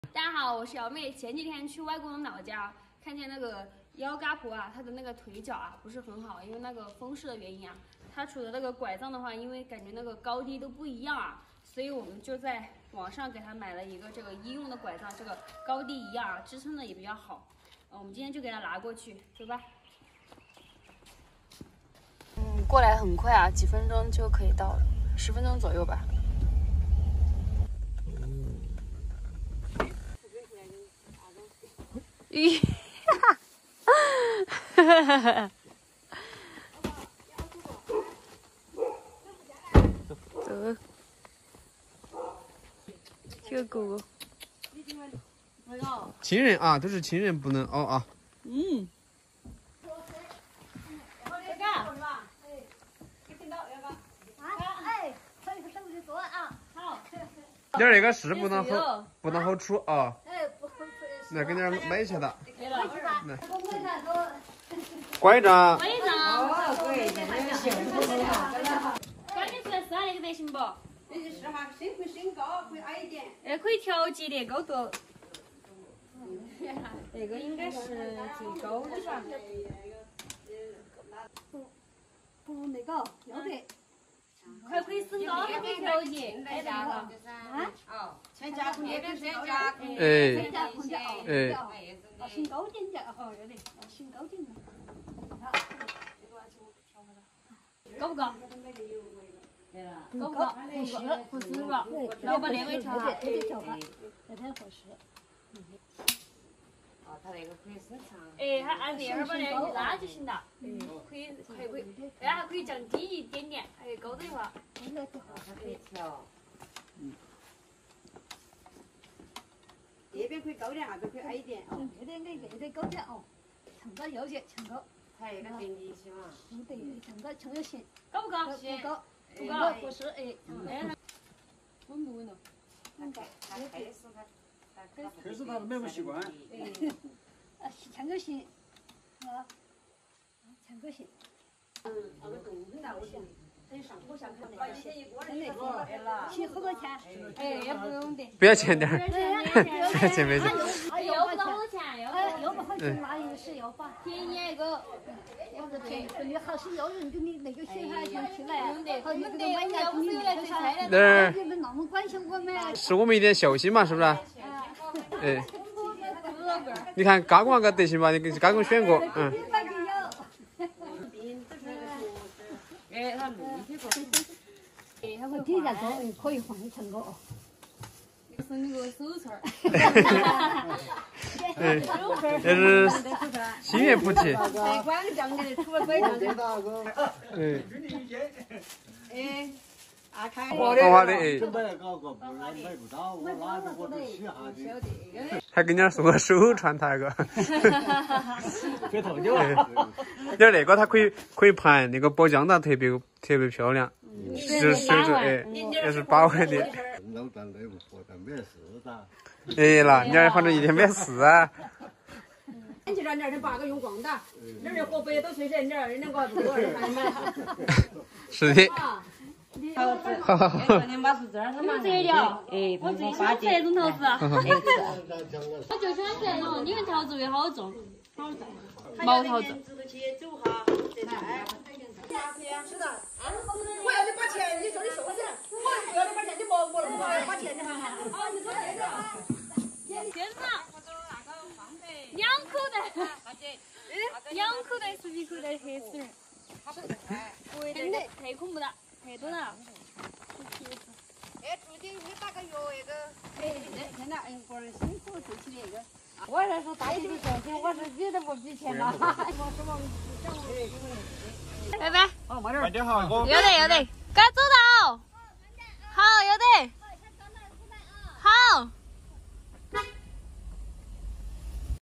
嘎公嘎婆腿脚不便，给他们买了新拐杖，临走非送两蛇皮袋蔬菜感谢【土家瑶妹】
上次来外公家，嘎公嘎婆腿脚不便，今天瑶妹特意给二老送了新拐杖~
欢迎大家来到瑶妹的频道！
有任何问题可+V沟通哦💖：jcc1995068
惊喜总在下一集💖😎